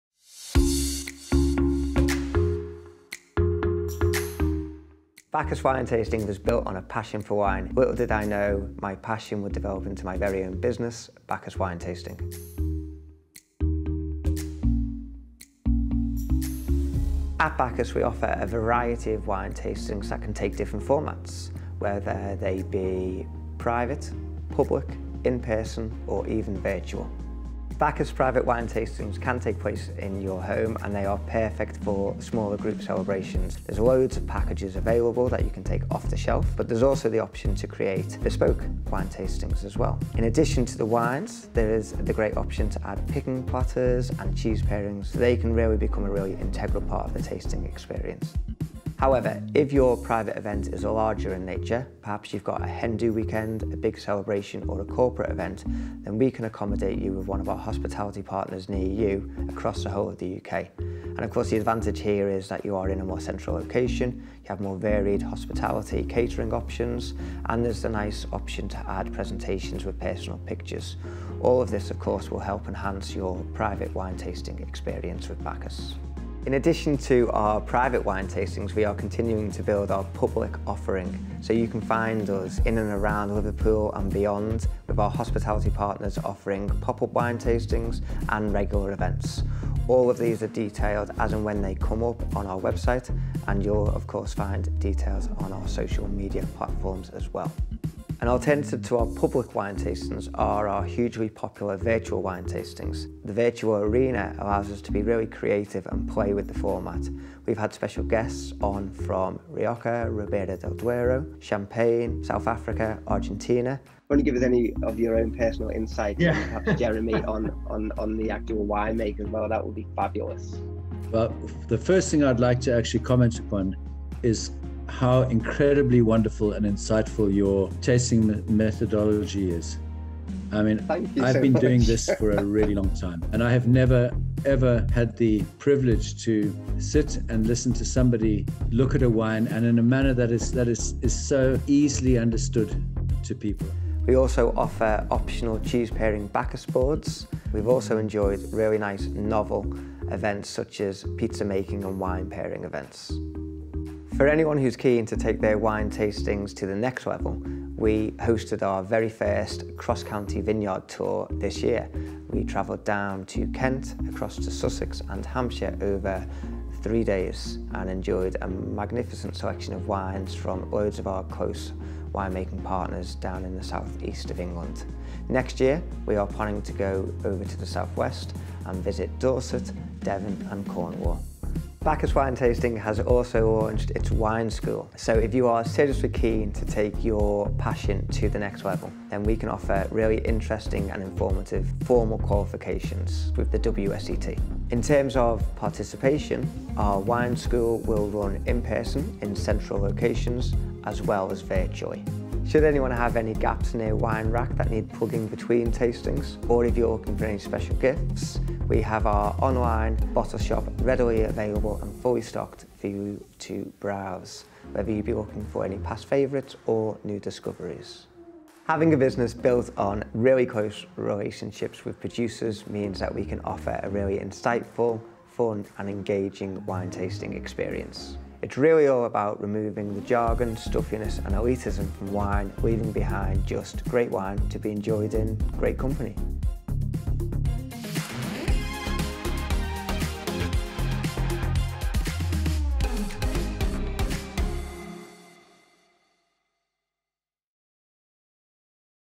Bacchus Wine Tasting: Our Showcase Video
Thank you for your interest in Bacchus Wine Tasting. In this longer video, you will find an overview of all aspects Bacchus Wine Tasting's offering. From small to large private wine tasting experiences, public events with our hospitality partners, our Wine School with the WSET, cross-county vineyard tours and bottle sales and gifts.

Our Showcase video is perfect for anyone hoping to find the perfect wine experience for a wine-lover; whether that be yourself or a loved one.

Explore our Youtube page for a selection of 'short' videos that drill down on specific aspects of our Business: wine tasting experiences, public events and vineyard tours.

We hope you enjoy finding out more about Bacchus Wine Tasting.

Thank you for your interest in Bacchus Wine Tasting,
Johnathan